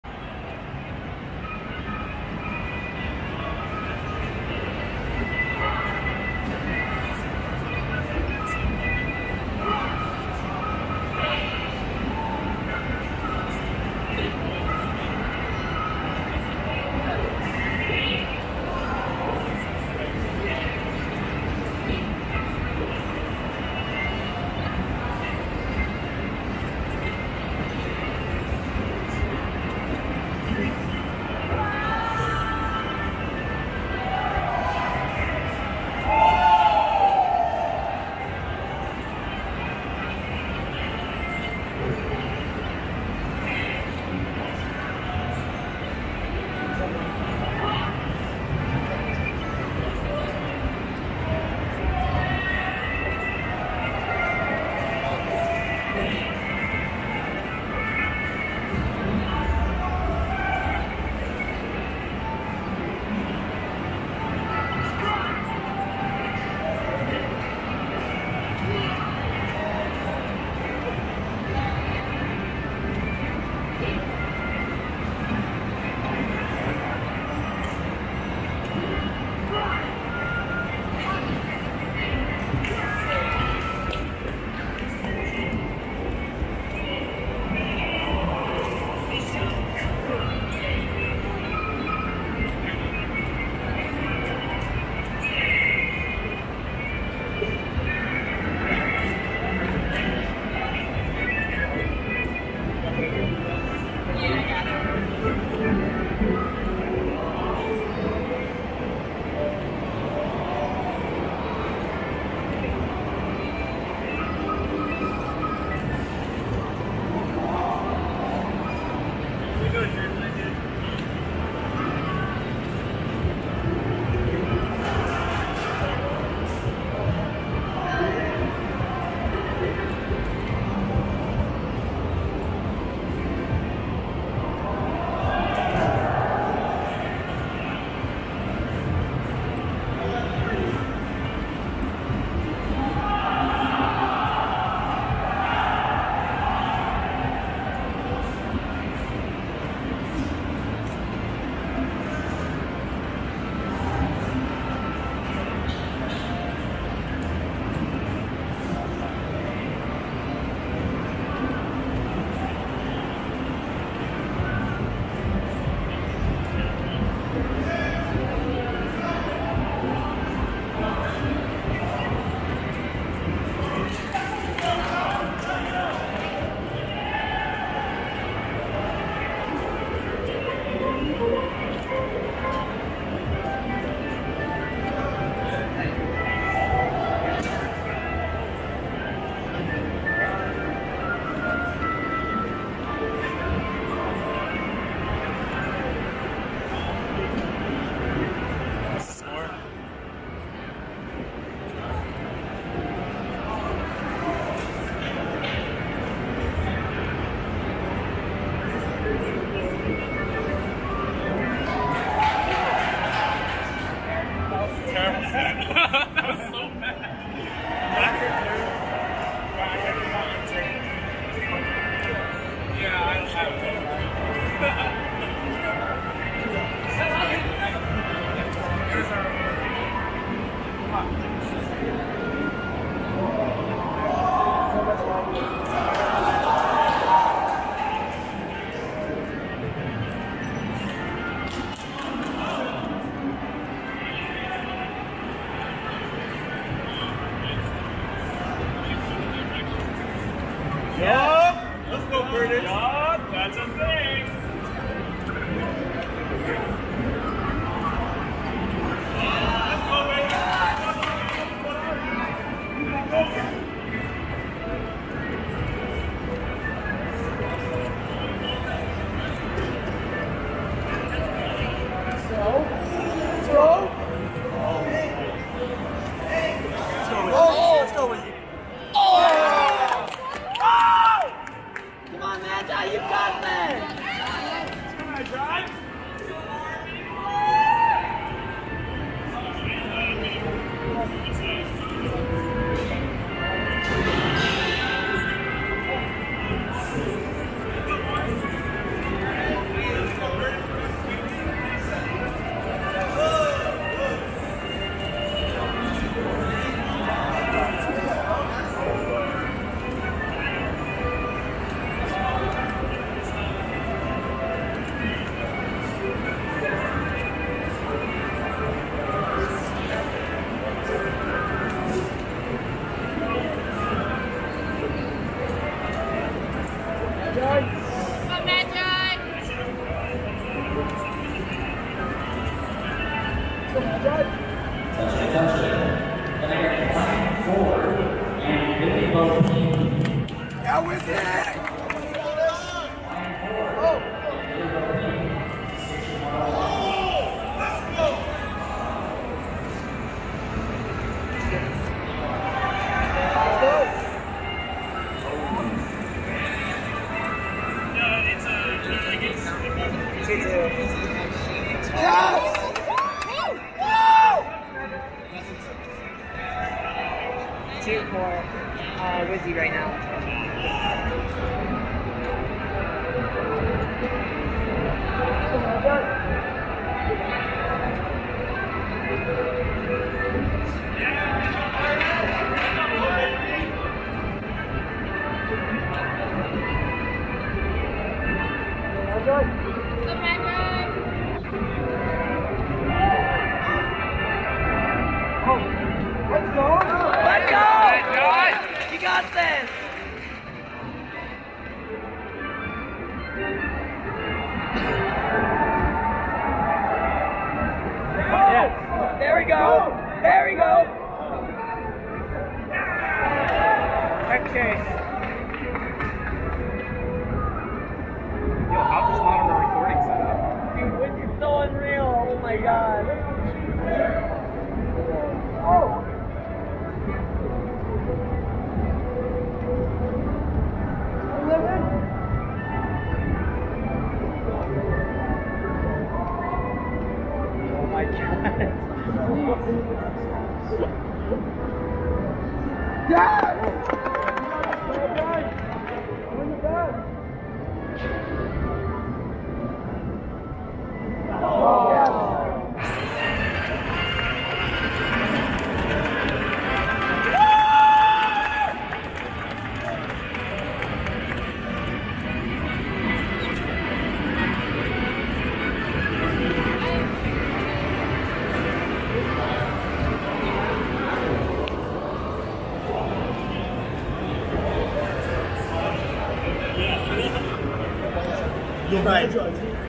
Leffen, Mango, Wizzrobe, Lucky | Top Players in action | Big House 9
A walk around Big House 9 during Top 16.. lots of action and sightings of top players, including off stream matches.